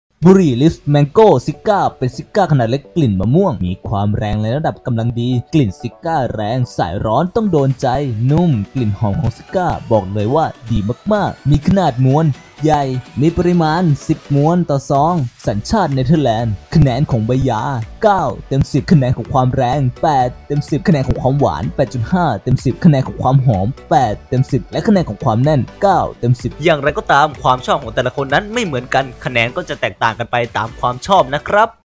บุหรี่ Lips Mango Cigars ลิป ชิก้าขนาดเล็กหวาน กลิ่นมะม่วง มวนใหญ่ สัญชาติเนเธอร์แลนด์
บุหรี่ Lips Mango Cigars
ซิก้าขนาดเล็กหวาน มวนใหญ่

บุหรี่ Lips Mango Cigars เป็นซิก้าขนาดเล็กกลิ่นมะม่วง มีความแรงในระดับกำลังดี กลิ่นซิก้าแรงสายร้อนต้องโดนใจ
นุ่มกลิ่นหอมของซิก้า บอกเลยว่าดีมากๆ

20 มวน/ซอง
สัญชาติ เนเธอร์แลนด์
ทำความรู้จักกันให้มากกว่านี้ได้ที่นี่ไลน์@
หรือ ติดตามข่าวสารเพิ่มเติม